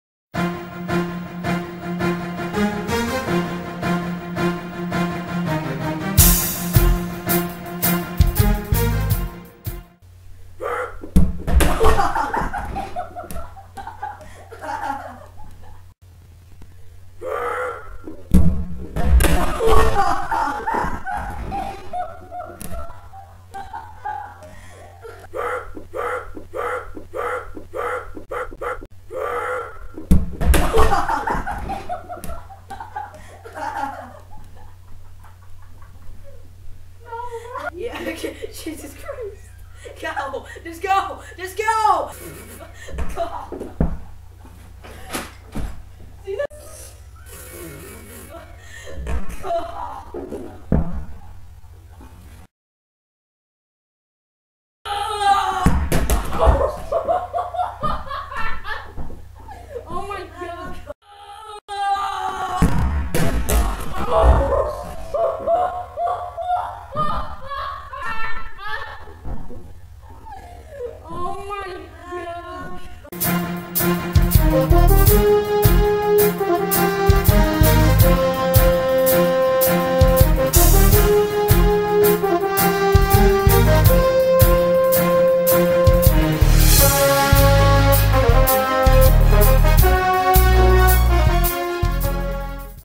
Big Ball Headshots!
So me and my friend made a game called the big red ball. What you do is on one person runs towards the ball and when the runner is close enough, someone kicks it at him